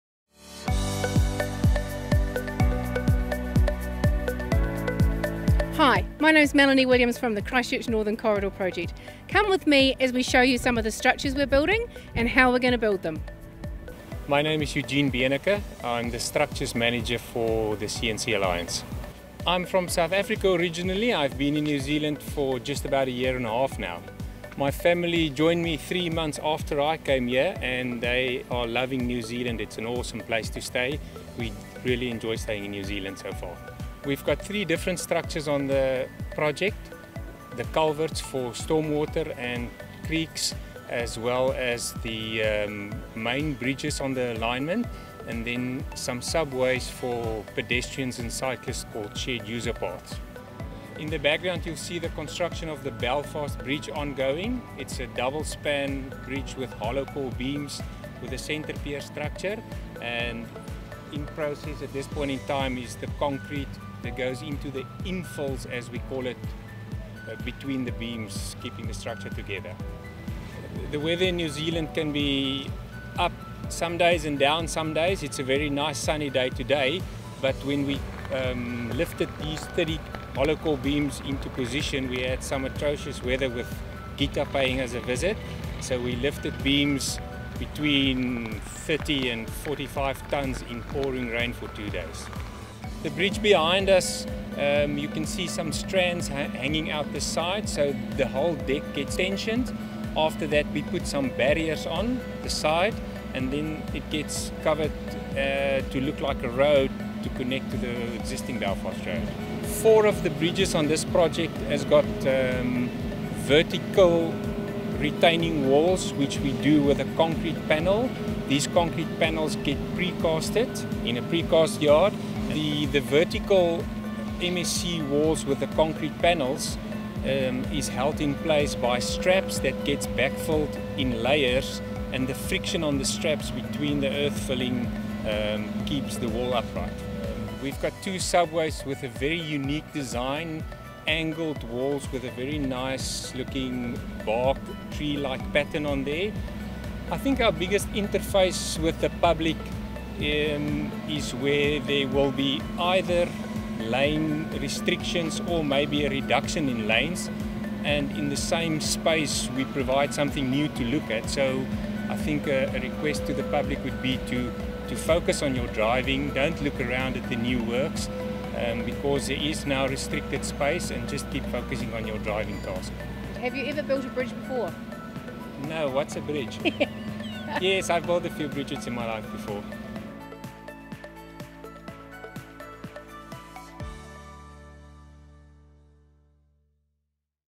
Christchurch Northern Corridor (CNC) – Building big
Watch the CNC team’s structures manager Eugene Beneke talk about all the bridges, subways and culverts to be built on this major project.